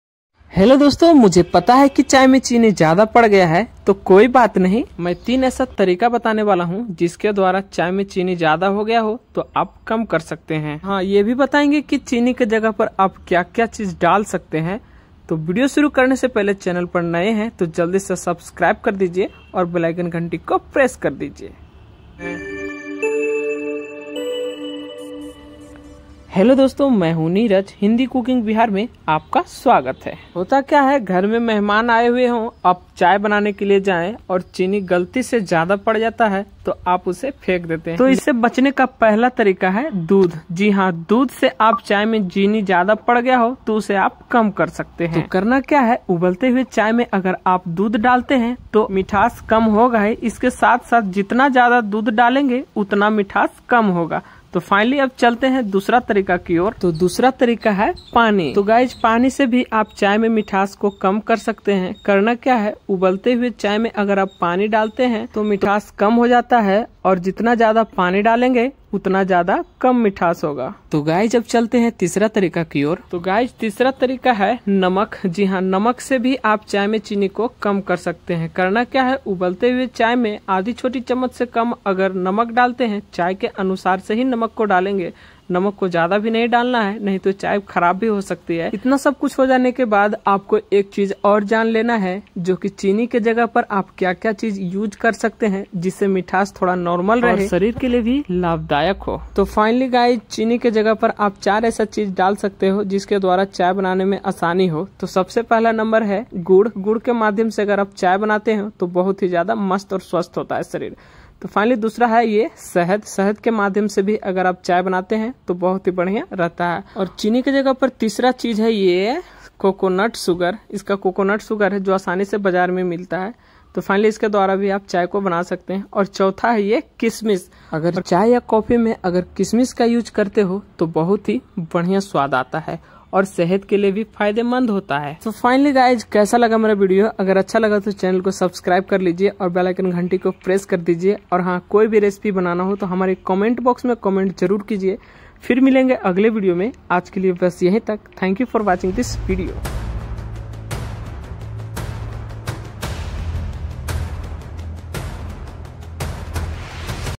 चाय में मिठास को कम कैसे करे || सिक्रेट तारिका || चीनी के जगह पर आप क्या डाल सकते।। chai video
चाय में चीनी को का करने का 3 सिक्रेट तारिका 😯😯 और चीनी के जगह पर आप क्या डाल सकते।। chai video